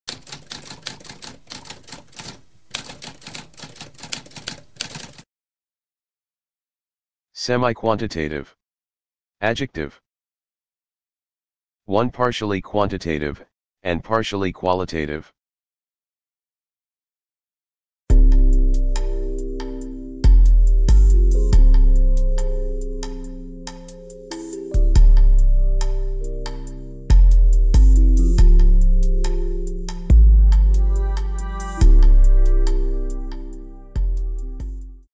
What does semiquantitative mean?
A spoken definition of semiquantitative.

Intro Sound:
Typewriter - Tamskp